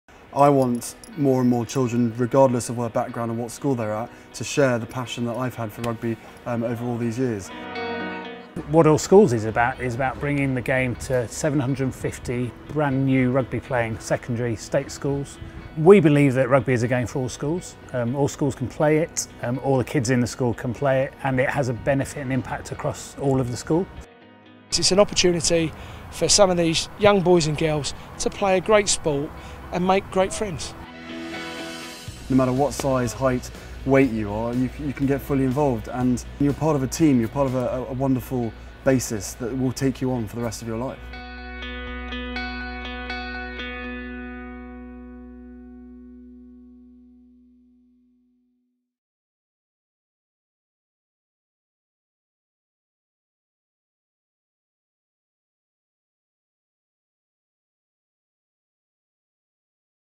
Prince Harry and Jason Leonard talk about 'All Schools'
Prince Harry and Jason Leonard explain what makes rugby great and why it's important to support 'All Schools' aim to grow rugby and get more children from all backgrounds playing and involved in rugby.